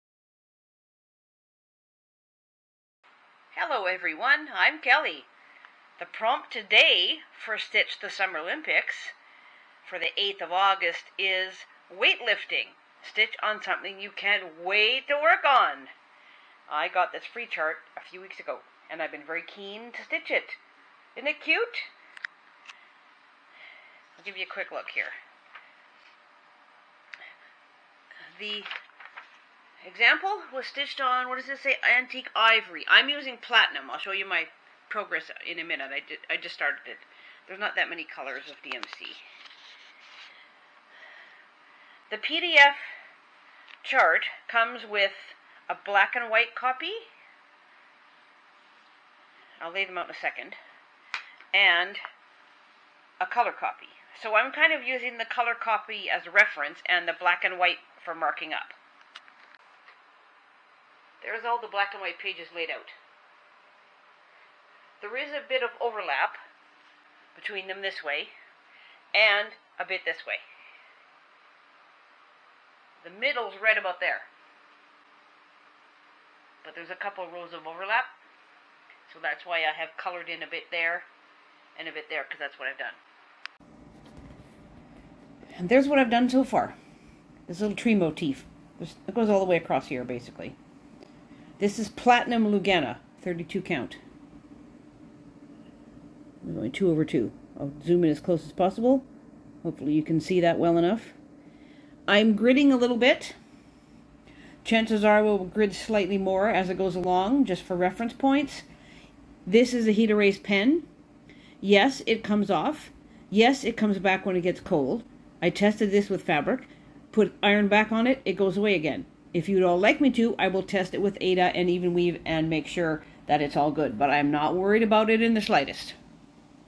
Stitch the Summer Olympics - 8 August
I have been wanting to start this free chart since I got it a few weeks ago. But today's prompt for "WEIGHT LIFTING - stitch something that you can't weight to stitch" was good for this project.